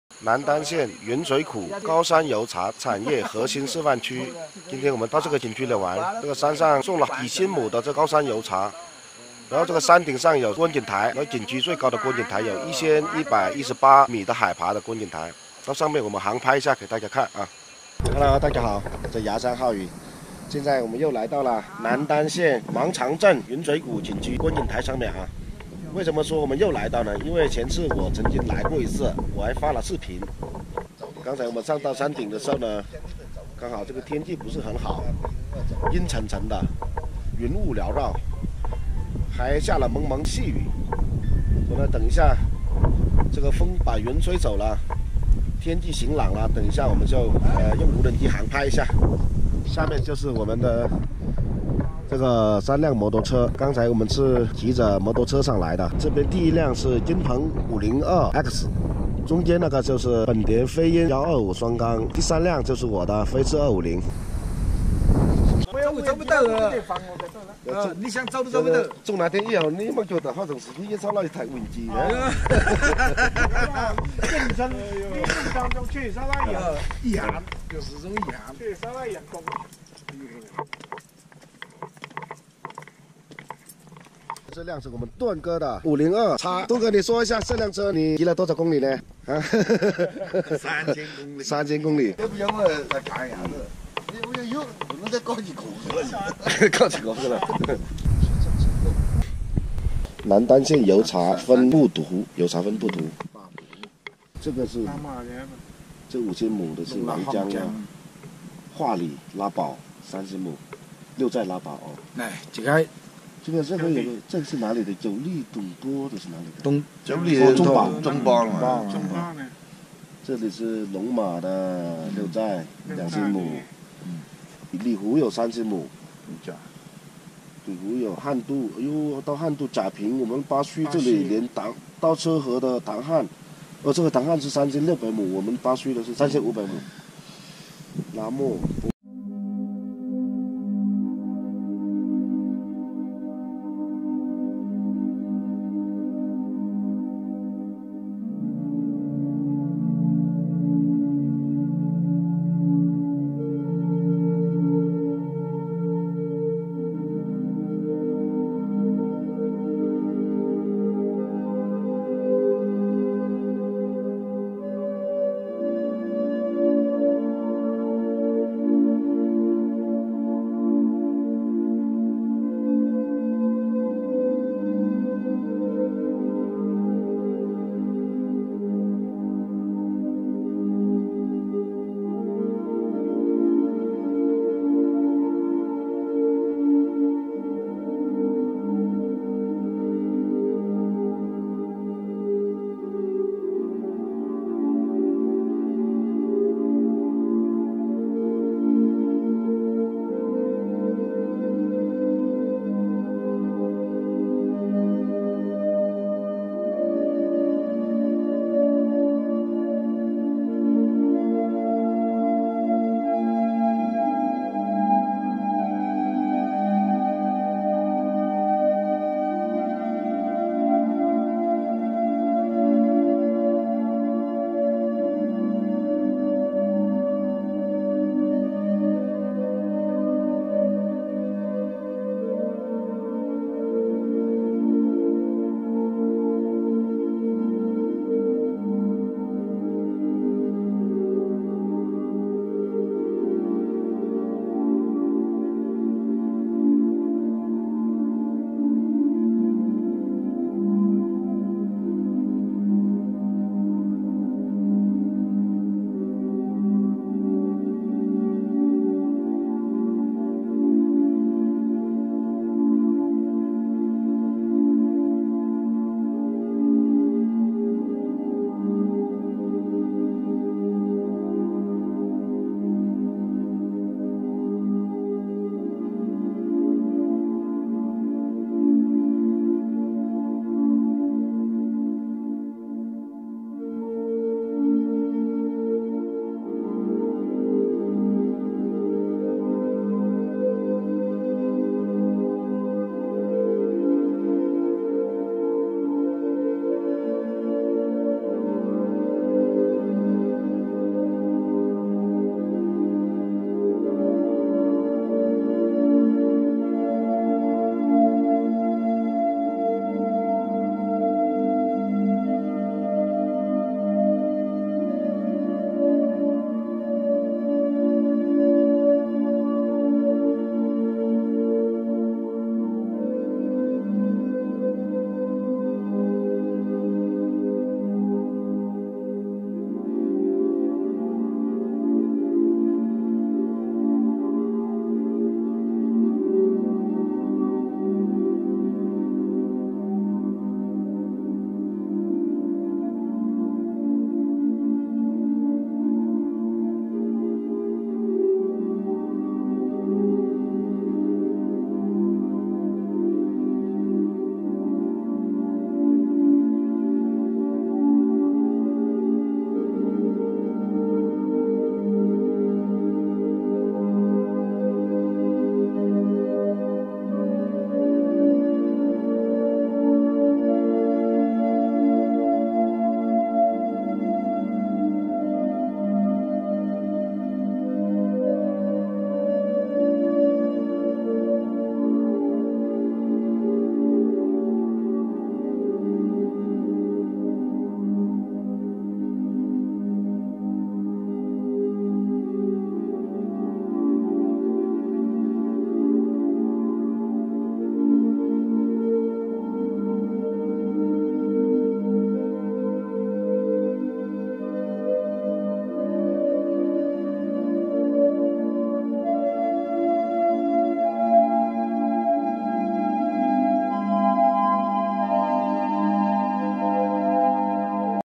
南丹县云水谷景区，登顶正值云雾缭绕，游客执意航拍，结果如何？有雾的时候更是一种难得的秘境啊！
Yunshui Valley Scenic Area in Nandan county, when the top is surrounded by clouds, tourists are determined to take aerial photos. What's the result? When there is fog, it is a rare secret place!